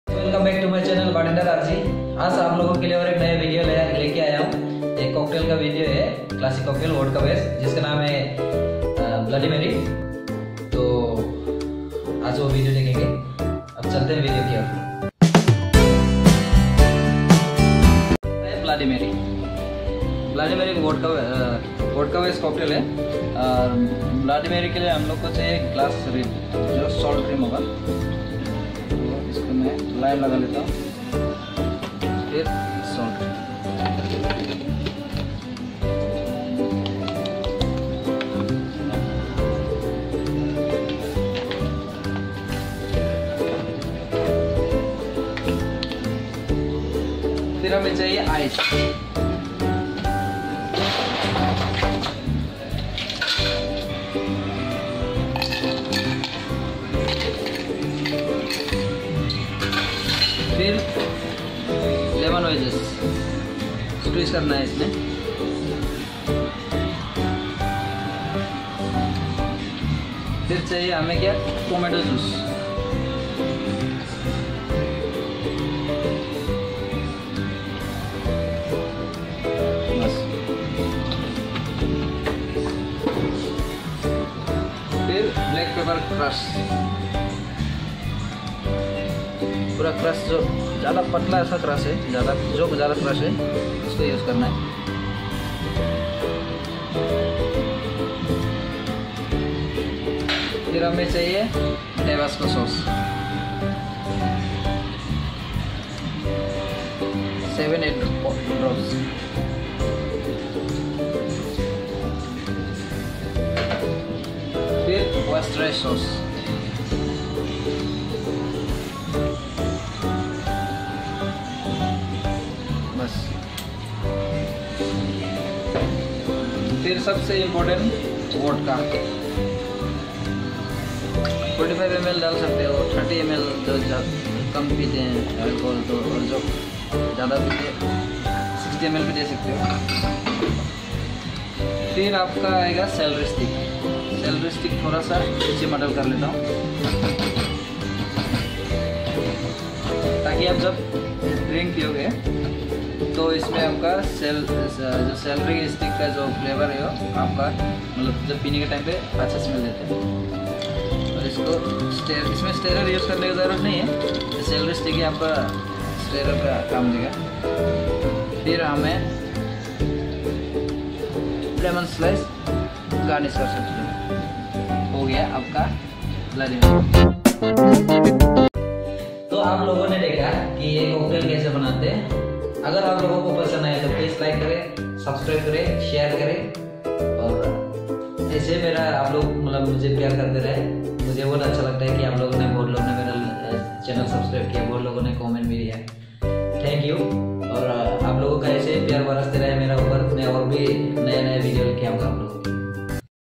How To Make Bloody Mary Cocktail
Bloody Mary Cocktail is a classic cocktail made of vodka, tomato juice, Tabasco sauce, and some more spices flavour of salary herb's. It was invented in the years of 1920 or 1030.